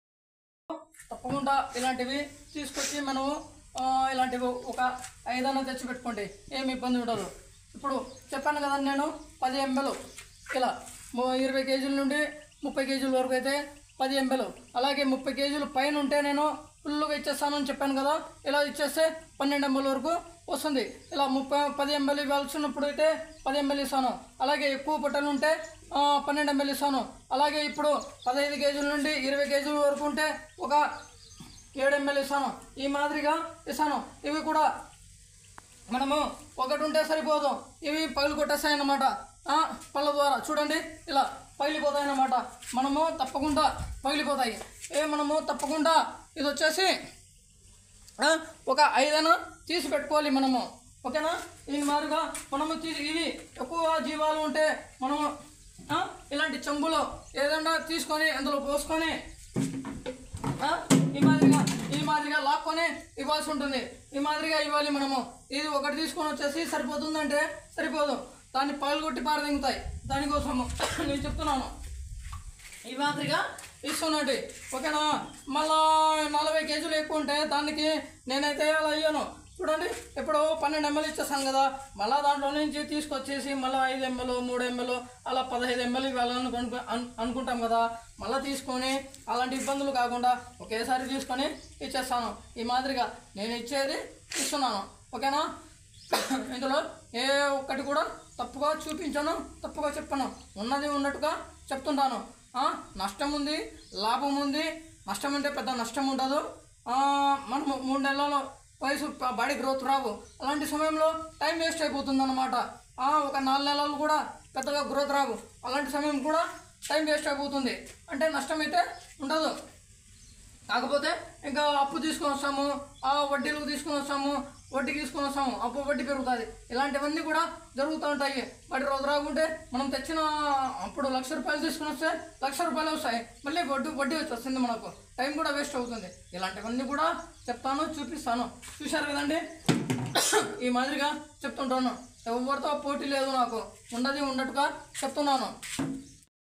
పొట్టేళ్లు, మేకలు కు నేను లివర్ టానిక్ ఇలా తాపుతాను|Liver tonic dosage
నేను పొట్టెల్లు, మేకలు కు లివర్ టానిక్ ఇలా తాపుతాను తప్పకుండా తాపించాలి